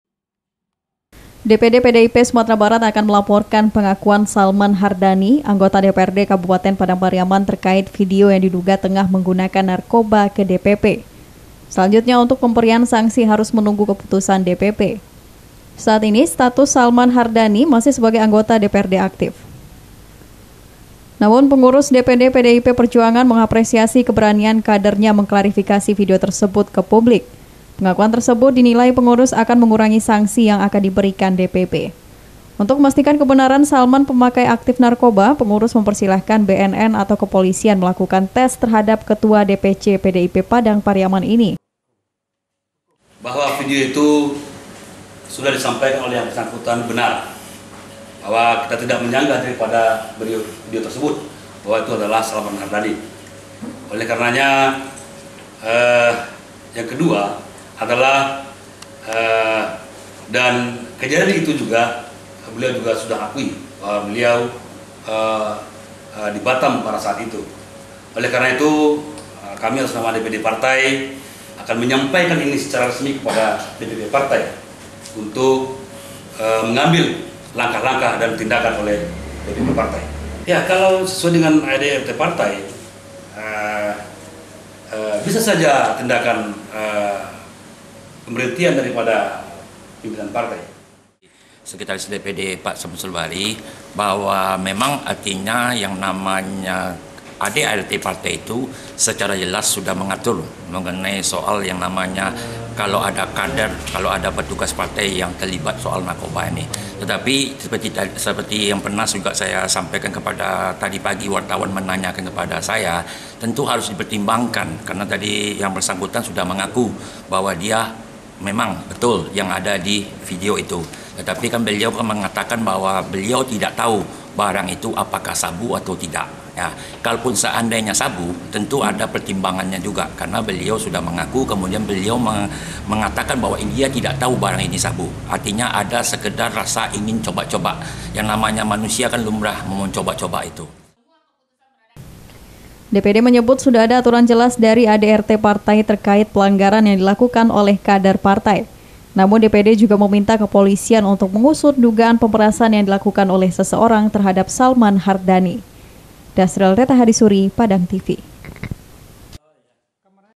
Kasus N4rkoba DPRD : DPD Serahkan Sikap Partai Kepada DPP PDI-P
DPD PDI-P sumbar menyerahkan  persoalan vidio dugaan narkoba salah satu kadernya di dprd kabupaten padang pariaman kepada dpp pdi-p. Sebelum ada keputusan dpp, kader tersebut masih berstatus sebagai anggota dprd padang pariaman.